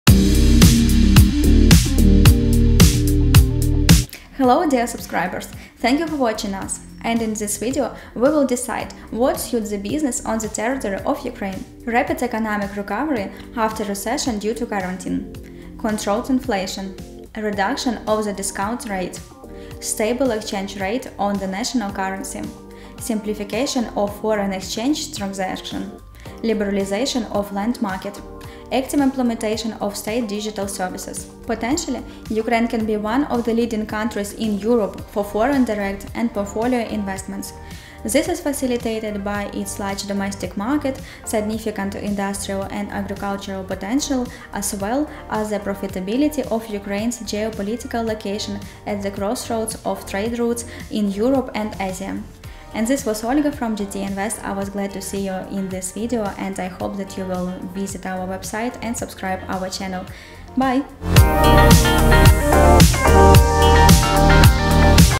What suits the business on the territory of Ukraine. Investing in Ukraine. GT Invest Ukraine.
- Key-factors of business leading on the territory of Ukraine
- Favorable conditions for investments in Ukraine
- Ukrainian potential

Call to GT Invest and I'll be happy to answer your questions.

GT Invest - your operator of business processes in Ukraine